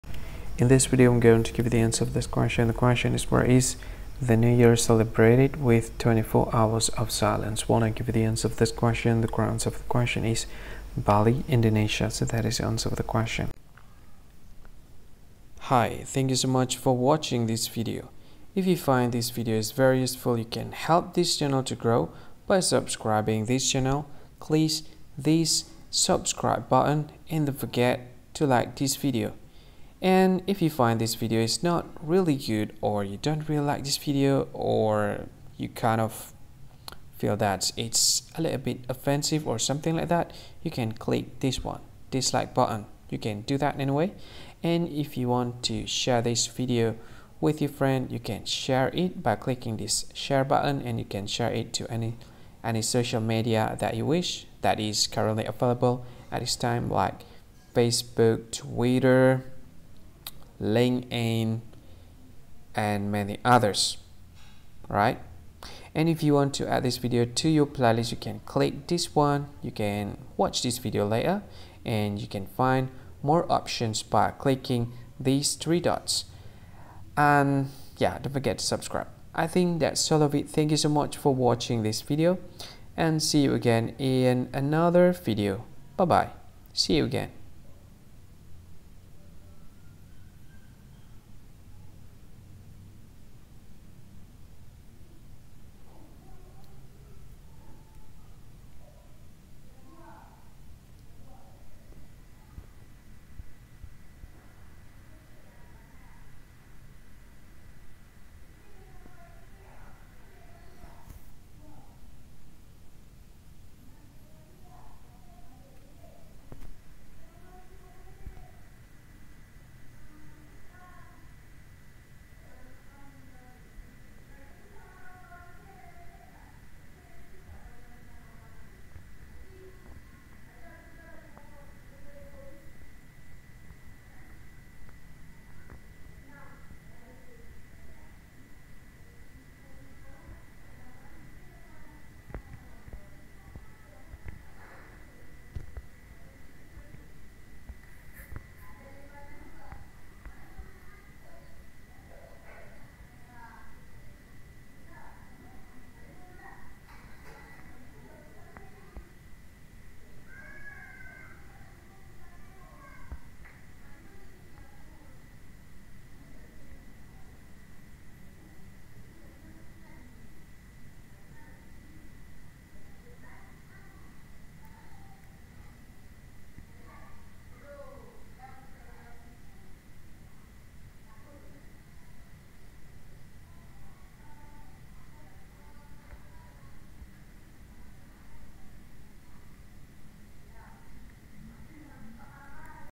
Where is the new year celebrated with 24 hours of silence?
Where is the new year celebrated with 24 hours of silence? NEW VIDEO#